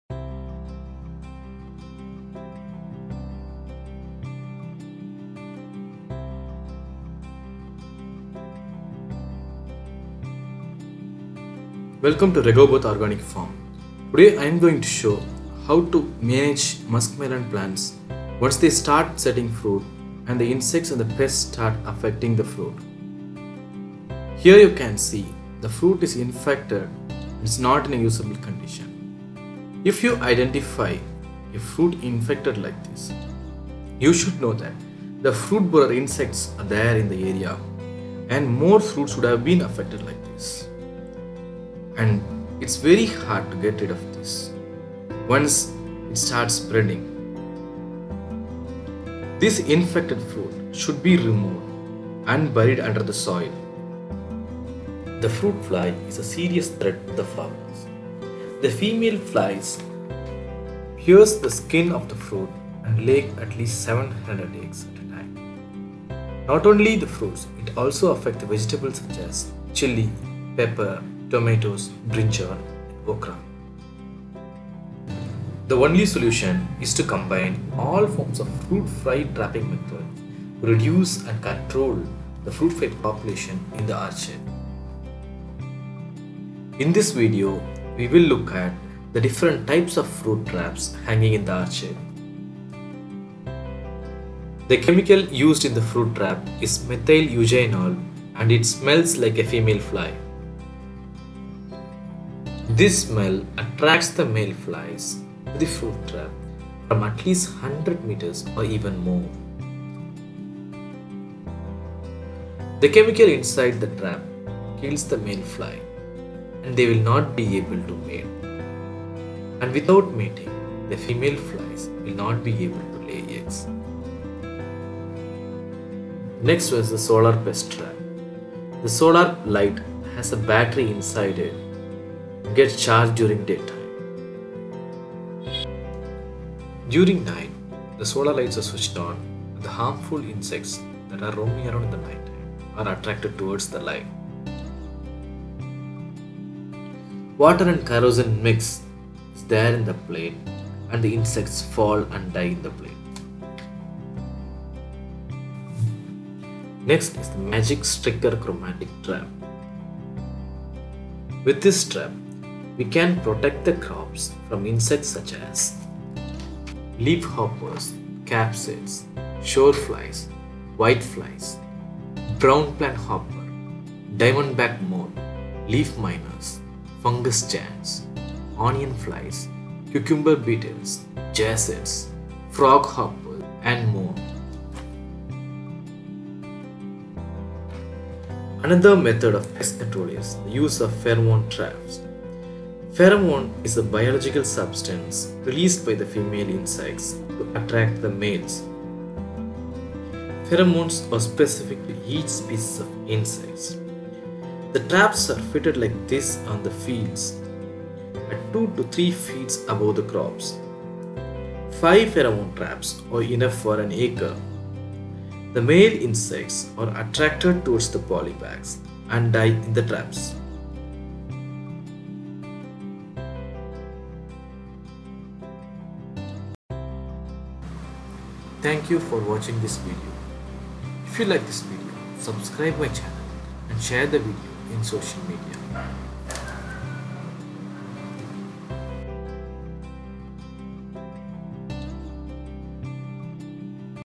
How to control pest in Muskmelon Cultivation / Organic pesticide control for Cantaloupe cultivation
In this video we will show how to control pests and rodents in  Cantaloupe / Muskmelon farm.